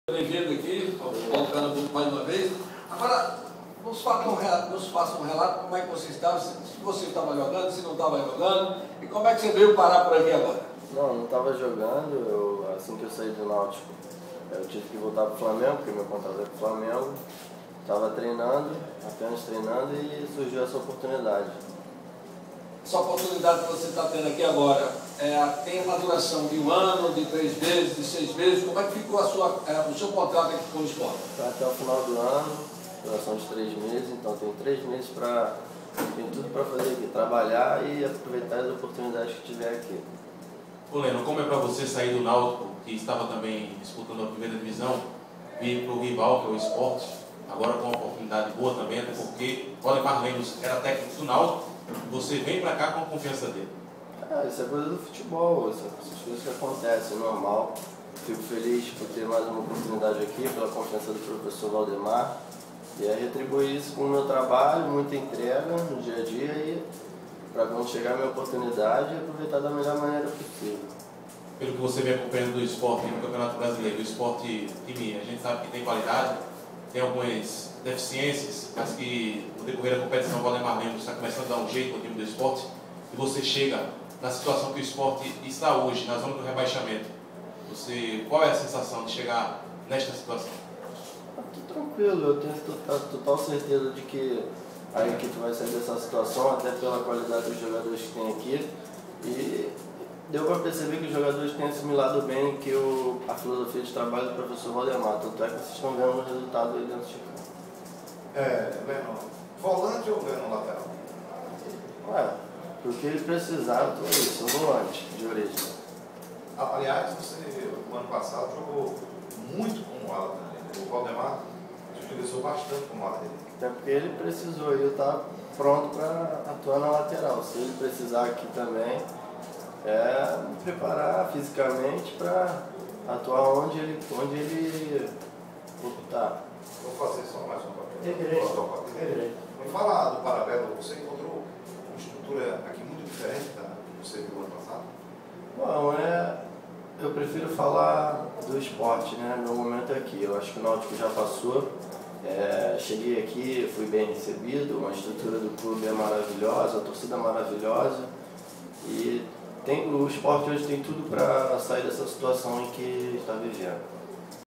Apresentação do volante Lenon
Jogador concedeu primeira entrevista coletiva